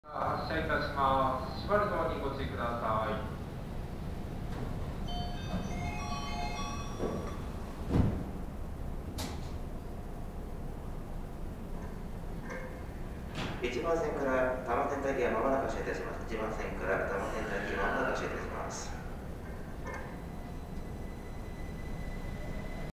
多摩都市モノレール1000系多摩センター行を撮った。
多摩センター駅にて